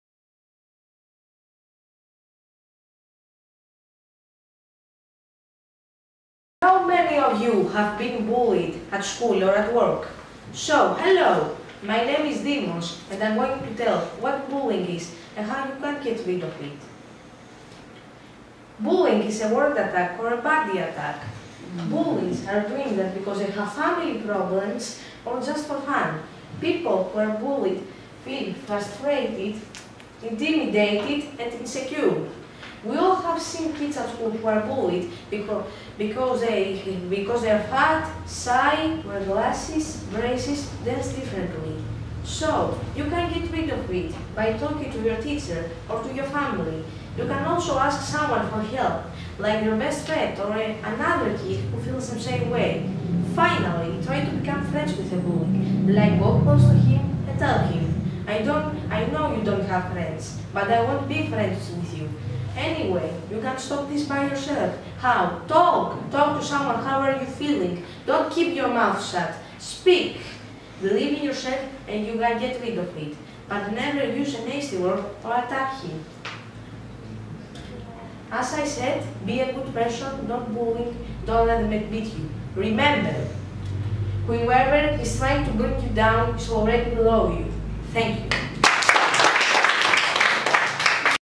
Bullying can be beaten | Dimos Karvelis | Zachariou FLS
Bullying  is constantly present in our life.  Dimos Karvelis gives advice on what to do and how to deal with bullying. The main point he focuses on is to speak up.

Have you ever been bullyied? Listen to Dimos Karvelis’ advice on how to deal with it and help others face it , too.

Facilitator: IRENE ZACHARIOU